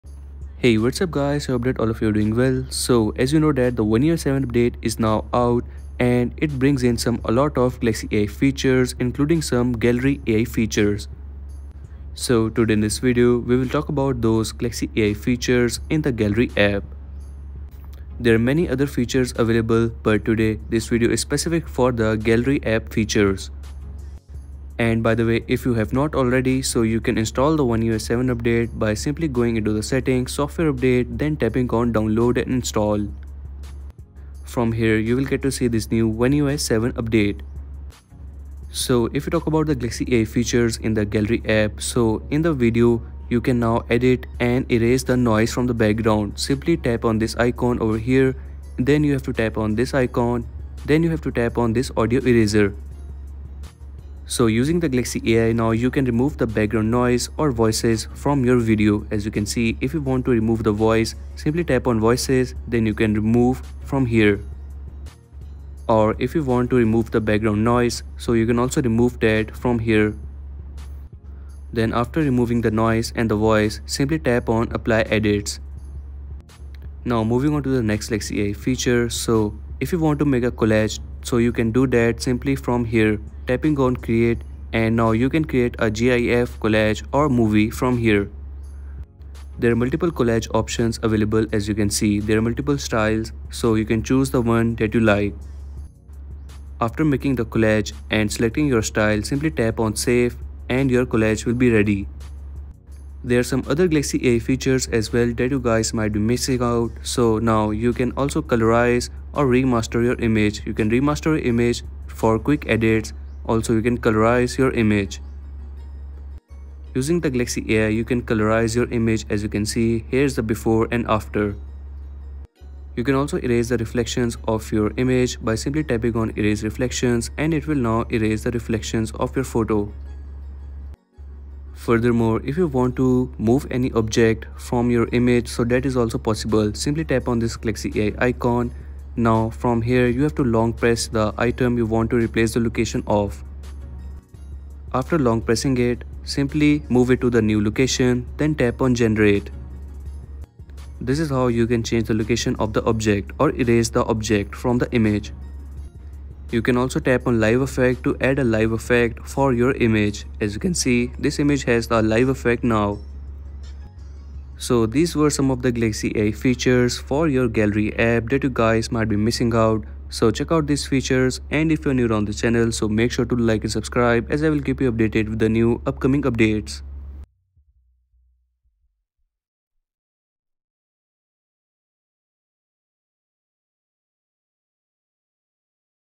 S24 Ultra: One UI 7's NEW Gallery AI! You WON'T Believe These Features!
So try out these Ai features for your Gallery app on your Galaxy s24 ultra